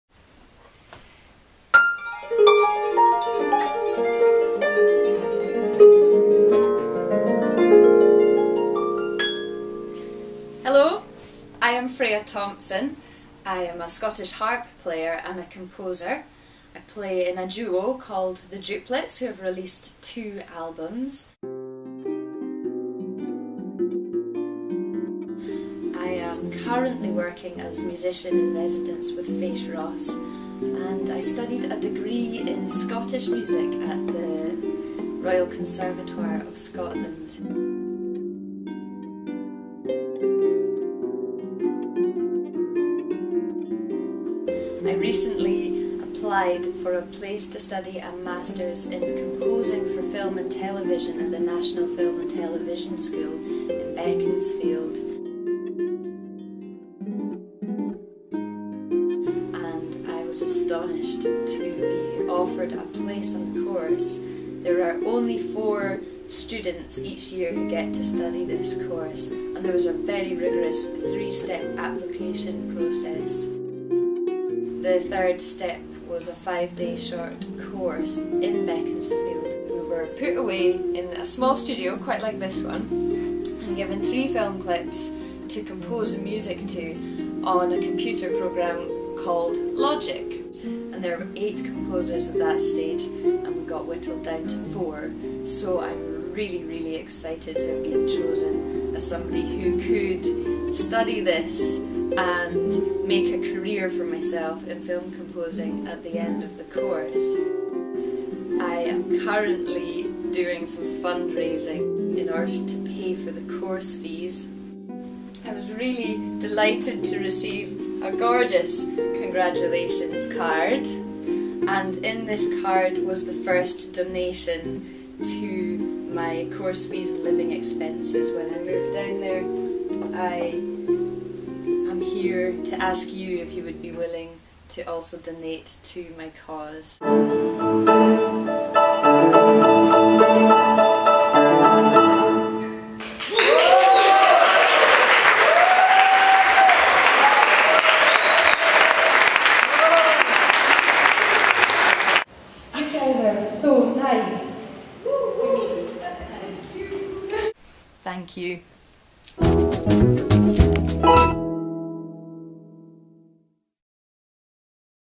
Sponsume - Fraya Thomsen
Please sponsor my place at the National Film and Television School!